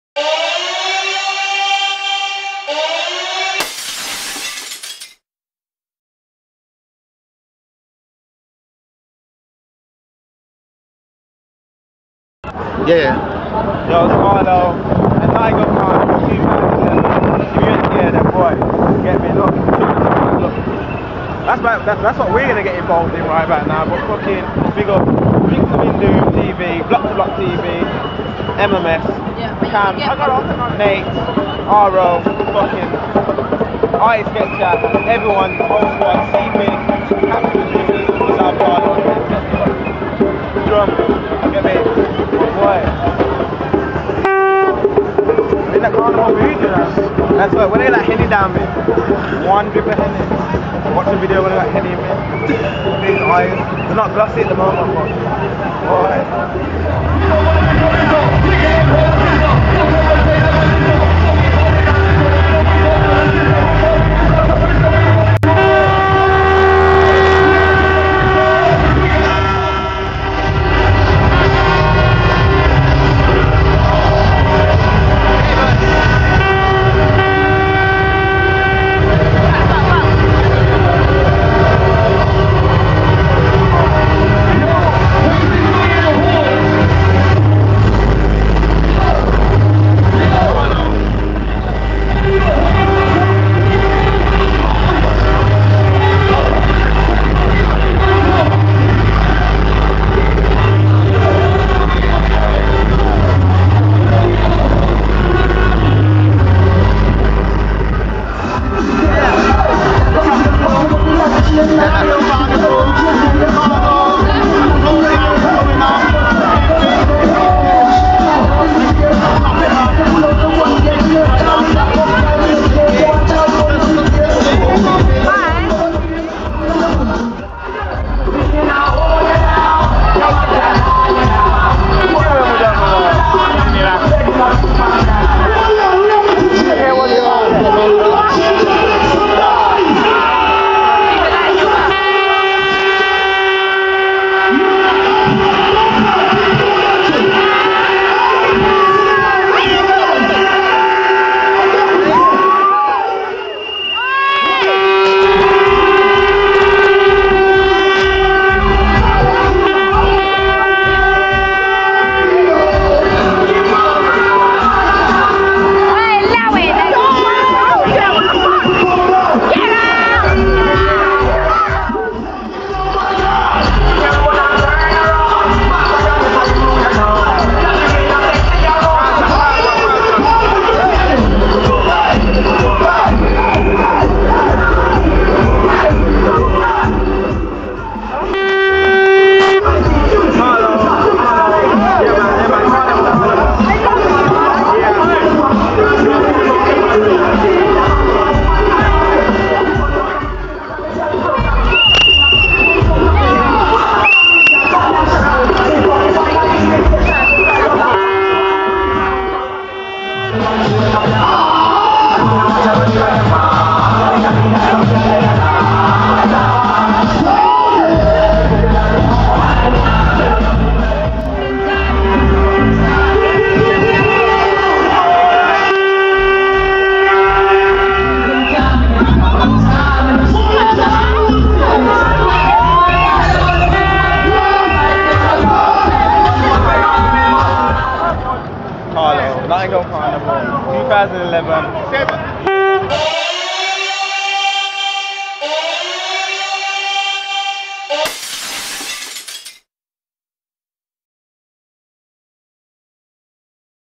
Notting Hill Carnival 2011 - Tattoo Karlo & The Capatal
Notting Hill Carnival 2011 in London was anticipated to be Europe's Biggest carnival/festival. So Victor Vin Doom Tv decided to go down there and see what all the fuss was about, and booooy did we get to see!

Also on our travels; we met up with Tattoo Karlo & The Capatal for one crazy afternoon...It was an interesting one to say the least!

Follow Karlo on Twitter; @TattooKarlo
Follow The Capatal on Twitter; @thecapatal